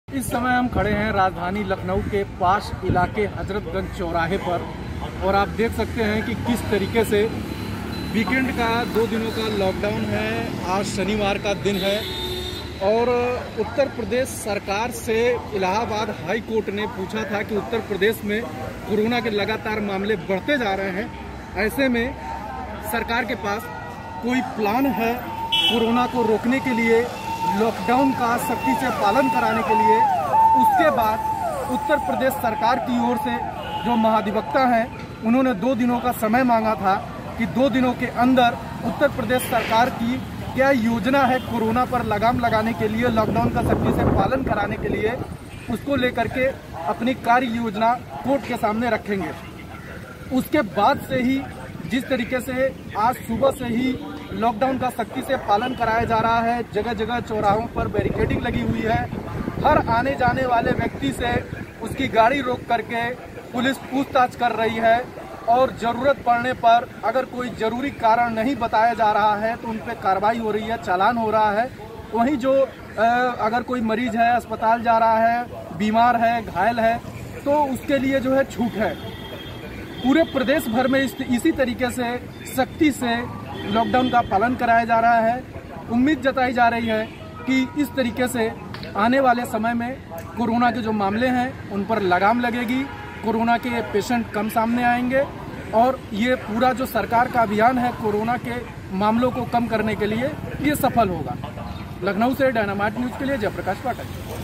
लखनऊ: यूपी में लॉकडाउन पर पुलिस की जबरदस्त सख्ती, कई सड़कों पर सन्नाटा
कोरोना संक्रमण से बचाव के लिये दो दिन के वीकेंड लॉकडाउन पर पुलिस का चारों ओर सख्त पहरा